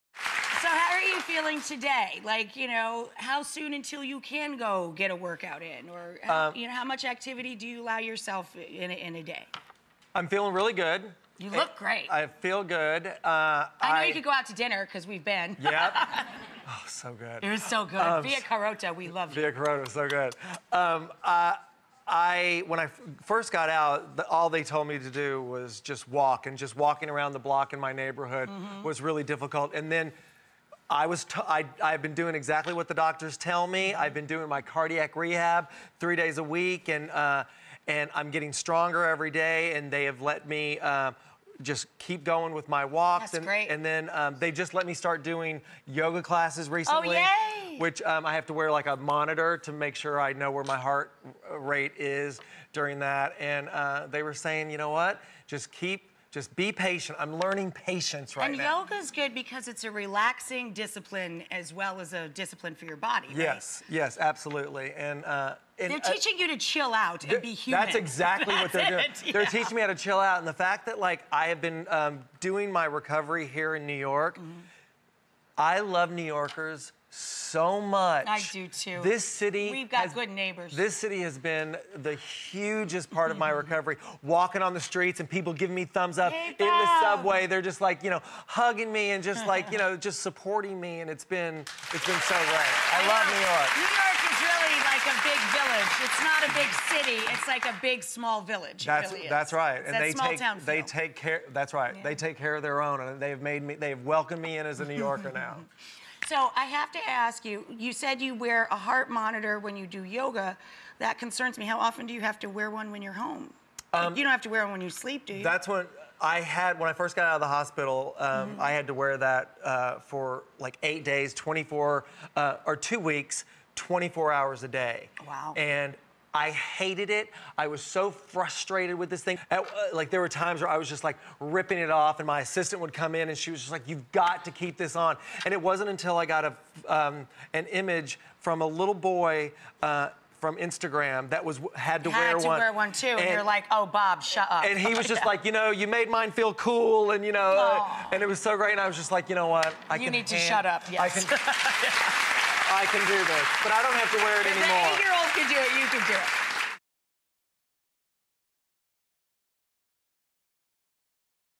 Bob Harper Talks About His Road to Recovery After His Near-Fatal Heart Attack
"I'm learning patience right now," says Bob Harper. Hear all about his recovery in the video.